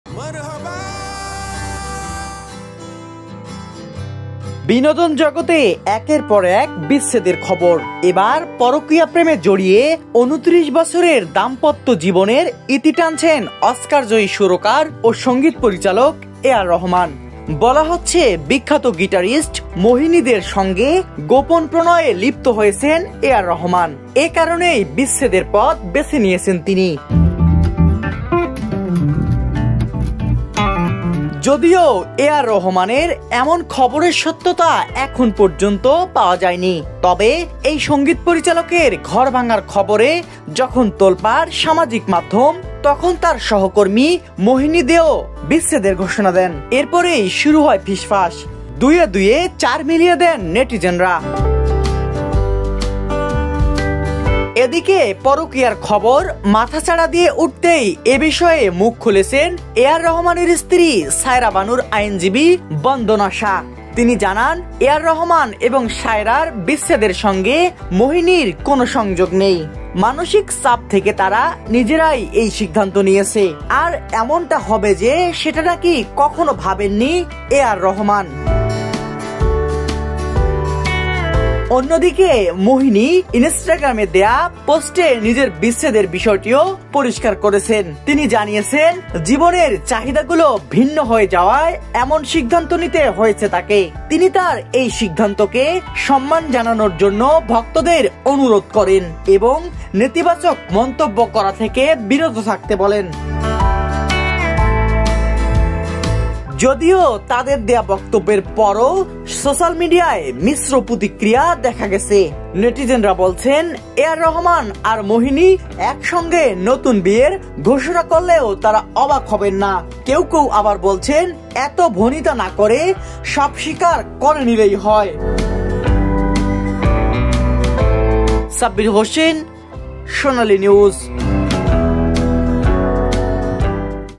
কে এই মোহিনী, যার সঙ্গে গোপন প্রেমে লিপ্ত এ আর রহমান? Mohini Dey | A R Rahman Divorce | Sonali News
বিনোদন জগতে একের পর এক বিচ্ছেদের খবর। এবার পরকীয়া প্রেমে জড়িয়ে ২৯ বছরের দাম্পত্য জীবনের ইতি টানছেন অস্কারজয়ী সুরকার ও সংগীত পরিচালক এ আর রহমান। বলা হচ্ছে, বিখ্যাত গিটারিস্ট মোহিনী দের সঙ্গে গোপন প্রণয়ে লিপ্ত হয়েছেন এ আর রহমান। এ কারণেই বিচ্ছেদের পথ বেছে নিয়েছেন তিনি।